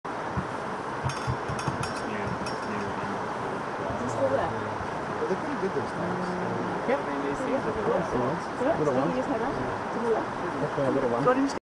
A Green Sea Turtle basking in the sun, Montgomery Reef, Western Australia
This is not a great video, but it's the only video I've got so far of a turtle. For some reason, Wordpress won't let me load it on my site (swpics.co.uk), so here it is. Not much to see, really; move on.